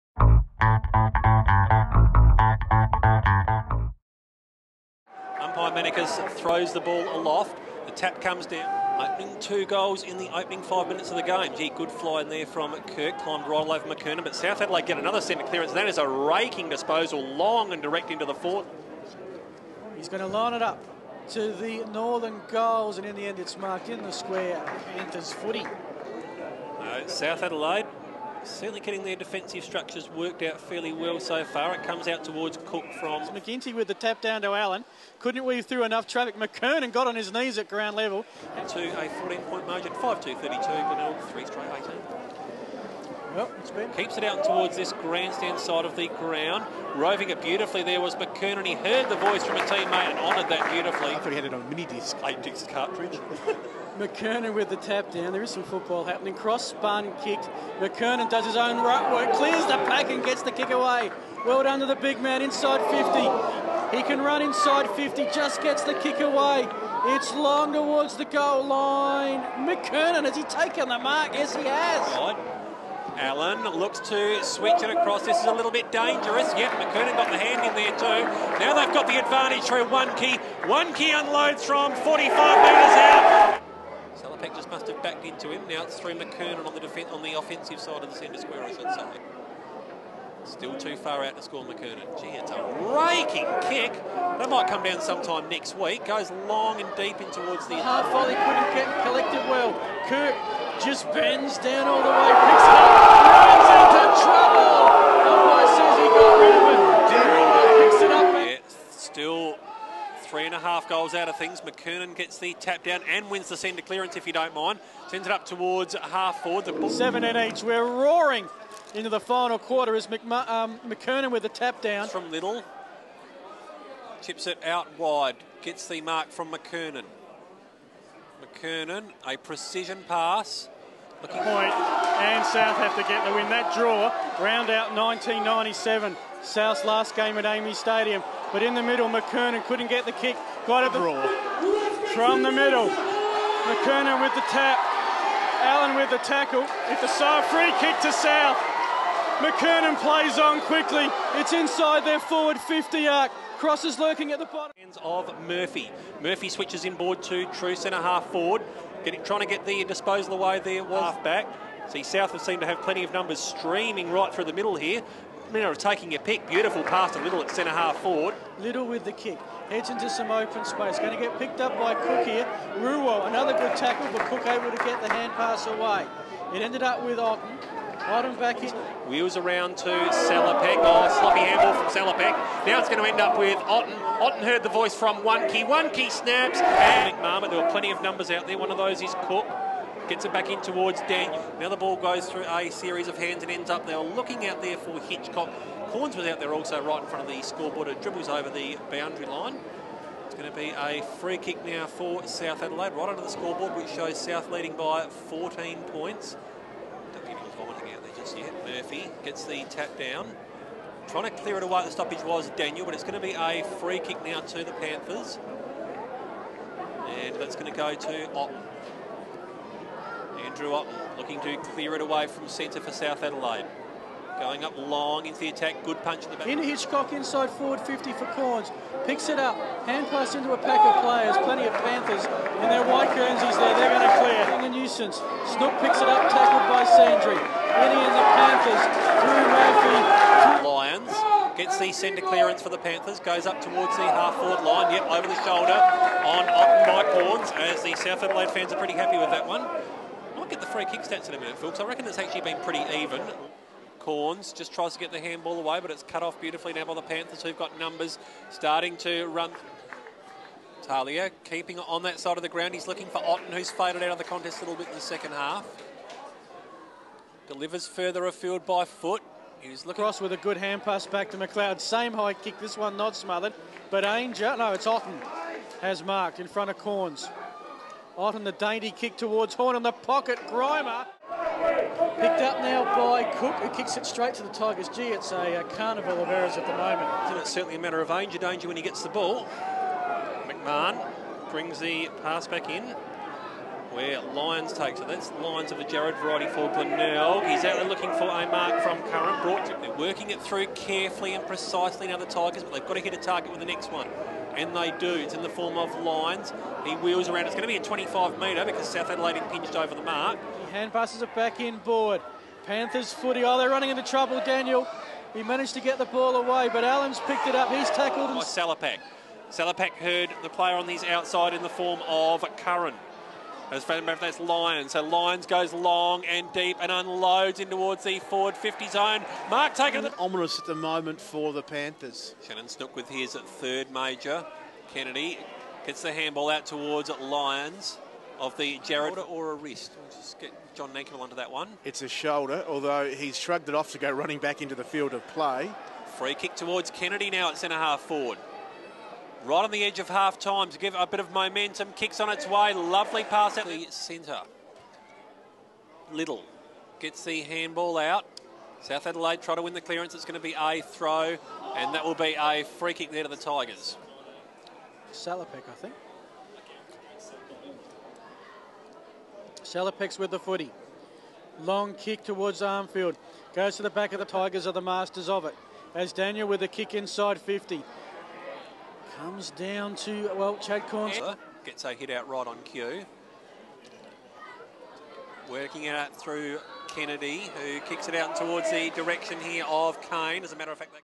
Crows in the SANFL: Round 10
Watch highlights of Shaun McKernan, Andy Otten and Jarryd Lyons.